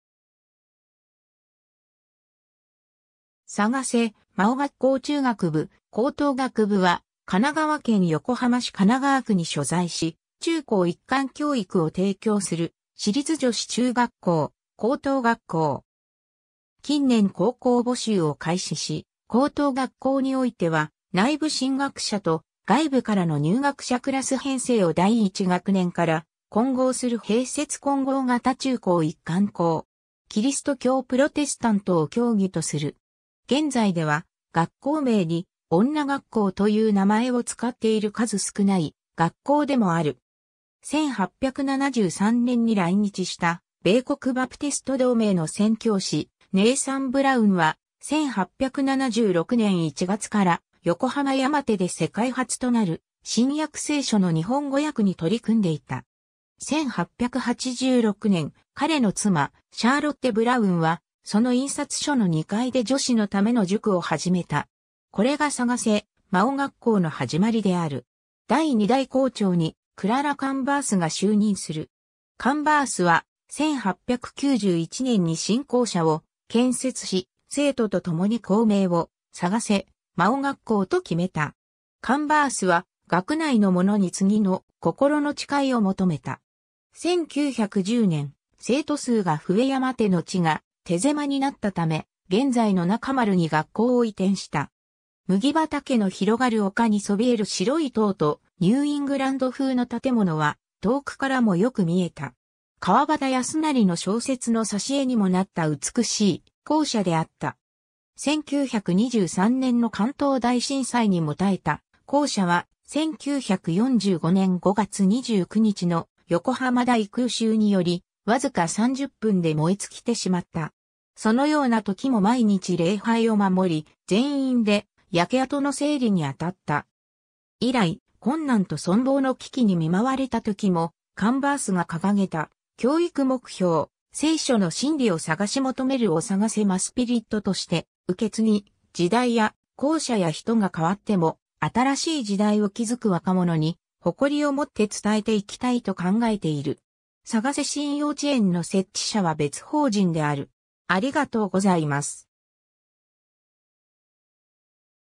捜真女学校中学部・高等学部
捜真女学校中学部・高等学部（そうしんじょがっこうちゅうがくぶ・こうとうがくぶ）は神奈川県横浜市神奈川区に所在し、中高一貫教育を提供する私立女子中学校・高等学校。
近年高校募集を開始し、高等学校においては内部進学者と外部からの入学者クラス編成を第1学年から混合する併設混合型中高一貫校。
 キリスト教プロテスタントを教義とする。
現在では学校名に「女学校」という名前を使っている数少ない学校でもある。
 1873年（明治6年）に来日した米国バプテスト同盟の宣教師ネイサン・ブラウンは、1876年1月から横浜山手で世界初となる新約聖書の日本語訳に取り組んでいた。
1886年、彼の妻シャーロッテ・ブラウンはその印刷所の2階で女子のための塾を始めた。
これが捜真女学校の始まりである。
 第2代校長にクララ・カンヴァースが就任する。
カンヴァースは、1891年に新校舎を建設し、生徒と共に校名を「捜真女学校」と決めた。
カンヴァースは学内の者に次の「心の誓い」を求めた。
 1910年（明治43年）、生徒数が増え山手の地が手狭になったため、現在の中丸に学校を移転した。
麦畑の広がる丘に聳える白い塔とニューイングランド風の建物は遠くからもよく見えた。
川端康成の小説の挿絵にもなった美しい校舎であった。
 1923年（大正12年）の関東大震災にも耐えた校舎は、1945年（昭和20年）5月29日の横浜大空襲により、わずか30分で燃え尽きてしまった。
そのようなときも毎日礼拝を守り、全員で焼け跡の整理にあたった。
 以来、困難と存亡の危機に見舞われたときもカンヴァースが掲げた教育目標「聖書の真理を捜し求める」を捜真スピリットとして受け継ぎ、時代や校舎や人が変わっても、新しい時代を築く若者に、誇りをもって伝えていきたいと考えている。
 捜真幼稚園（神奈川区栗田谷42-43）の設置者は別法人（学校法人捜真バプテスト学園）である。